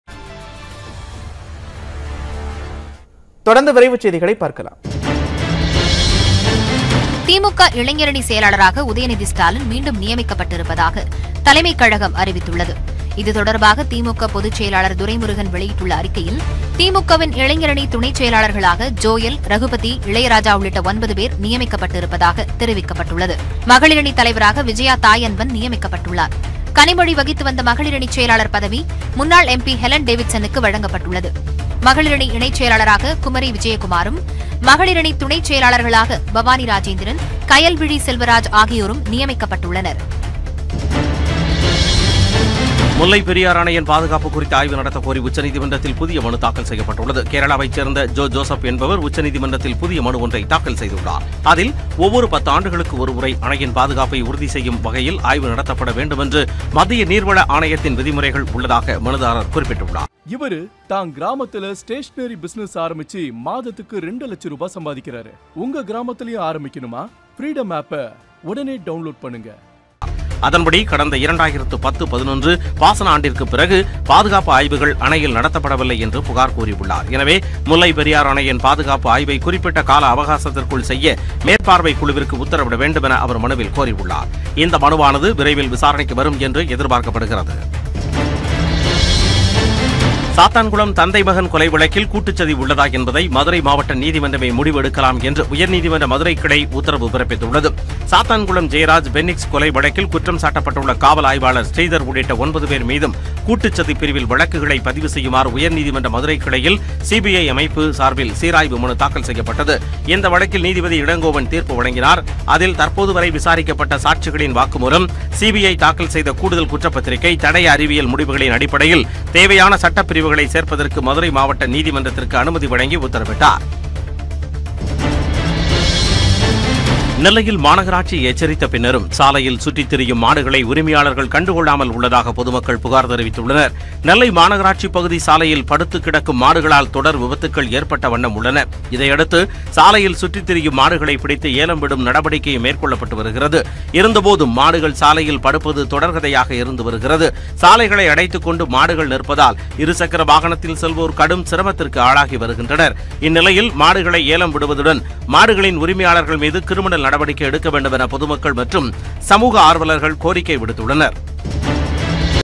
திமுகவில் புதிய பிரமுகர்களை நியமித்து பொதுச்செயலாளர் துரைமுருகன் உத்தரவு | Tamil News
திமுகவில் புதிய பிரமுகர்களை நியமித்து பொதுச்செயலாளர் துரைமுருகன் உத்தரவு | மீண்டும் இளைஞரணி செயலாளர் உதயநிதி | Tamil News

செய்திகளை துல்லியமாகவும் துரிதமாகவும் வழங்கும் தமிழகத்தின் முதன்மை செய்தி தொலைக்காட்சி உங்கள் நியூஸ் 18 தமிழ்நாடு நியூஸ்18தமிழ்நாடு news18tamilnadu TamilNews n18_Ad_Present